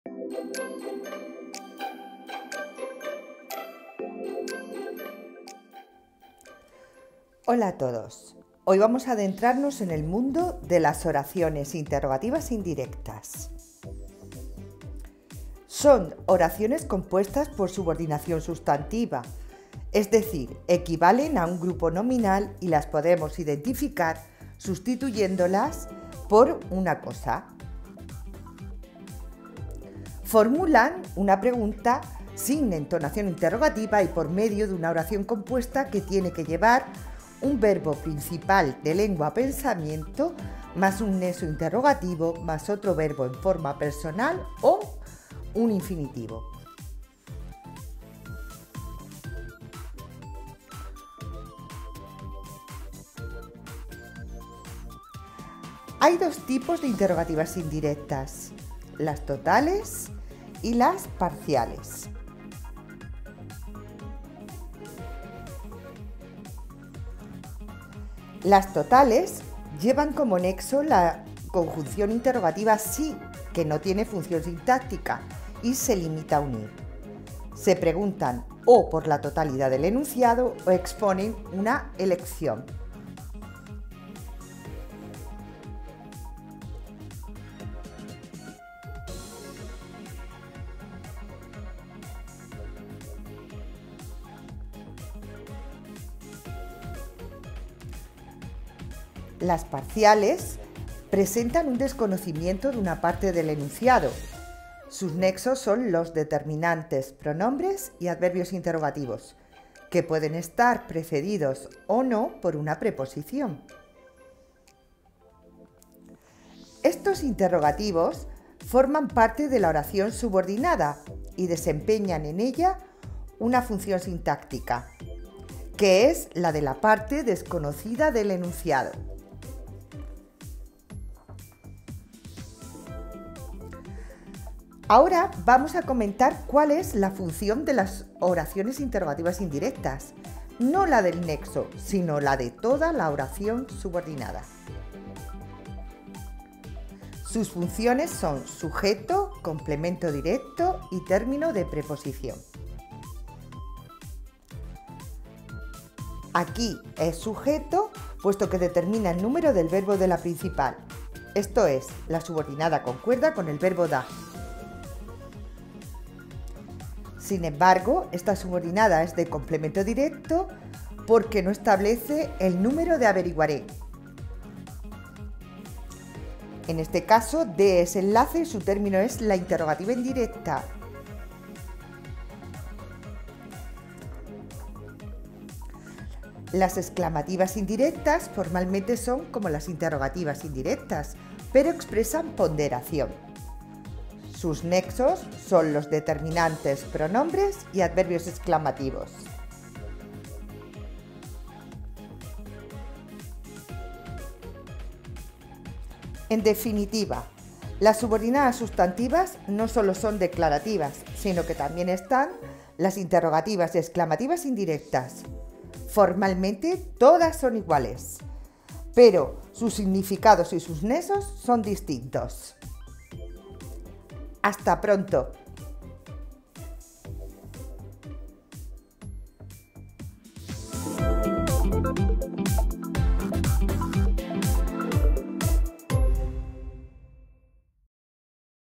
INTERROGATIVAS INDIRECTAS: OTRO TIPO DE SUBORDINADAS SUSTANTIVAS
Funciones, explicación y nexos de las subordinadas sustantivas interrogativas indirectas.